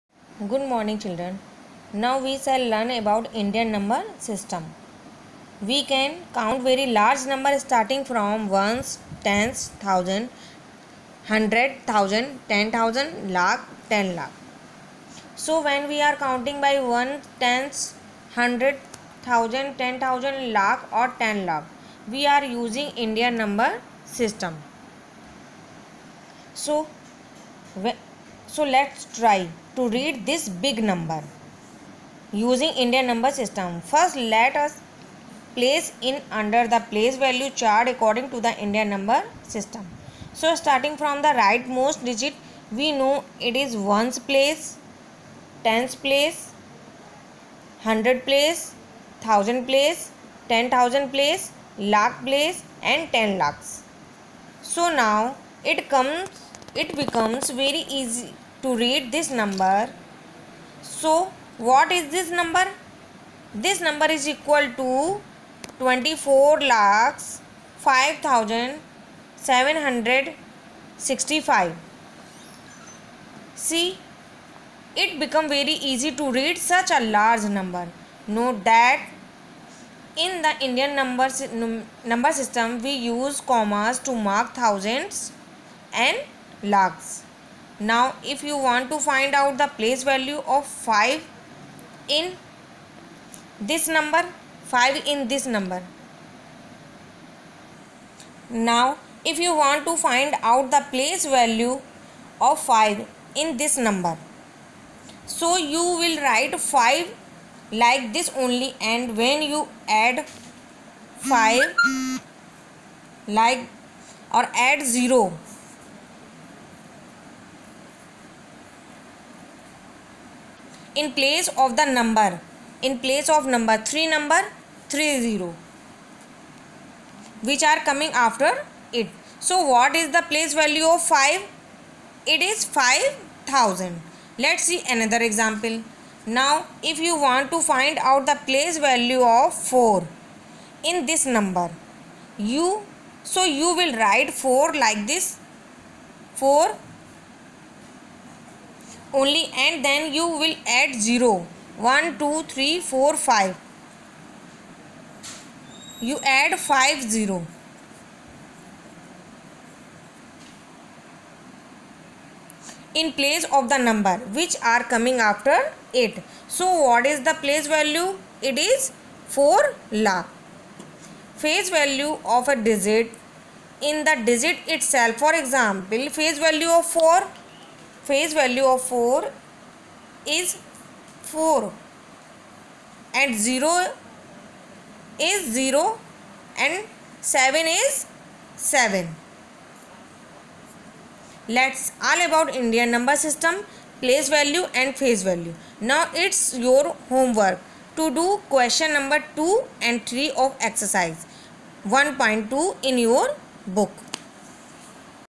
Indian Number System, Place Value and Face Value in very easy manner.
Indian Number System, Place Value and Face Value in very easy manner. Basically for class 3-5.